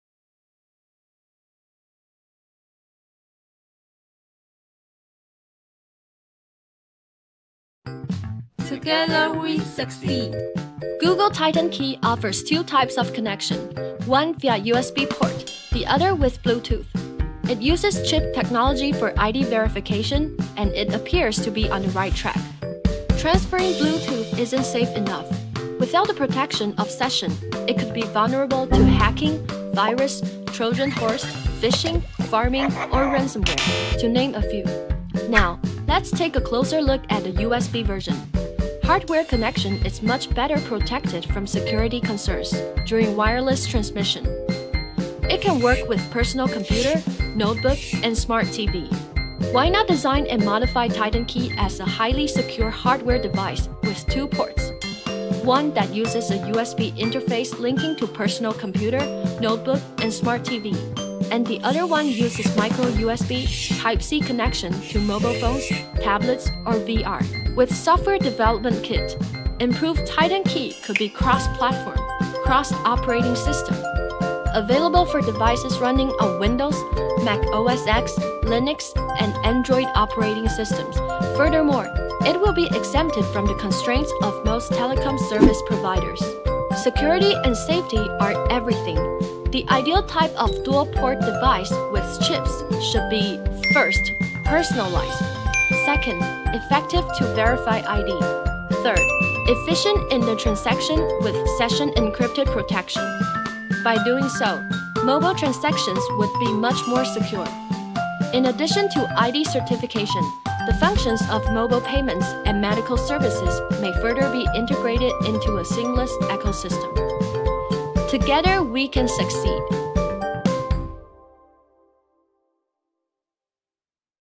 Google's TITAN KEY could be better together we succeed
Together we succeed
Google's Titan Key offers two types of connection,one via USB port, the other with Bluetooth.
It uses chip technology for ID verification and it appears to be on the right track.
Transferring Bluetooth isn’t safe enough.
Without the protection of “Session”, it could be vulnerable to hacking, virus, Trojan Horse, Phishing,
Pharming, or Ransomware, to name a few.
Now, let’s take a closer look at the USB version.
Hardware connection is much better protected from security concerns during wireless transmission.
It can work with Personal Computer, Notebook, and Smart TV.
Why not design and modify Titan Key as a highly secure hardware device with two ports,
one that uses a USB interface linking to Personal Computer, Notebook, or Smart TV,
and the other one uses Micro USB/Type C connection to mobile phones, tablets, or VR.
With Software Development Kit (SDK) “improved” Titan Key could be cross-platform,
cross-operating system (OS) available for devices running on Windows, Mac OS, Linux, and Android operating systems.
Furthermore it will be exempted from the constraints of most telecom service providers.
Security and safety are everything.
The ideal type of dual-port device with chips should be:
First, personalized
Second, effective to verify I.D.
Third, efficient in the transaction with Session Encrypted protection.
By doing so, mobile transactions would be much more secure.
In addition to ID certification, the functions of mobile payments and medical services may further be integrated into a seamless ecosystem.
Together we can succeed

一起合作會成功
Google推出新產品Titan Key，目前有兩種版本，一種是透過USB接口連接，另一種是透過藍牙無線網路連接。
使用硬體晶片做為身份認證，看似方向正確。
藍芽不是安全的傳輸方式，沒有搭配SESSION，網路容易遭受到駭客、病毒及木馬程式、網路釣魚等攻擊。
讓我們來看看USB的Titan Key，使用硬體的連接方式，沒有無線傳輸的安全顧慮，可以連接個人電腦、筆電、智能電視。
但是，Titan Key何不設計成一個硬體安全載具，搭配兩個讀寫頭，一邊是USB介面，可以連接個人電腦、筆電、智能電視，
另一邊是Micro USB/ Type C介面，就可以連接手機、平板、VR等移動裝置，如能搭配(SDK)，可達到跨平台、跨系統的功能，
可以在安卓, iOS, Windows, Mac OS…等系統使用，而且不受電信營運商箝制。
安全才是硬道理
理想的雙頭(介面)晶片載具必須經過個人化處理，提供身份認證，交易傳輸搭配Session加密防護，才能確保交易安全。
除了身份認證以外，支付、醫療等功能都可以整合於其中。